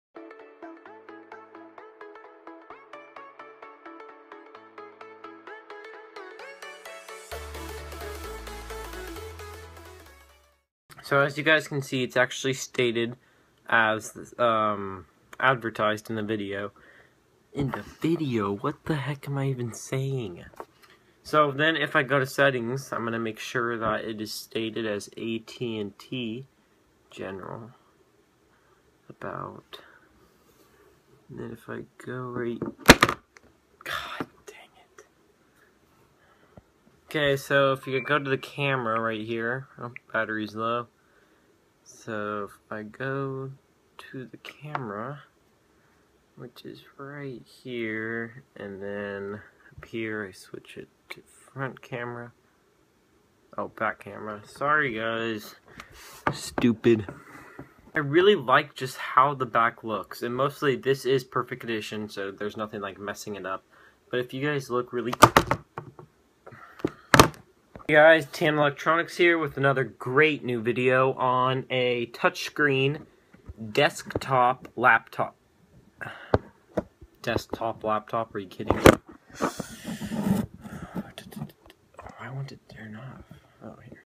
Bloopers!
Just some quick and funny bloopers of the videos I made today!

Use my code "XXMKVK" to join Mercari ( an app like eBay but with no fees!!!) and get $2 off your first purchase!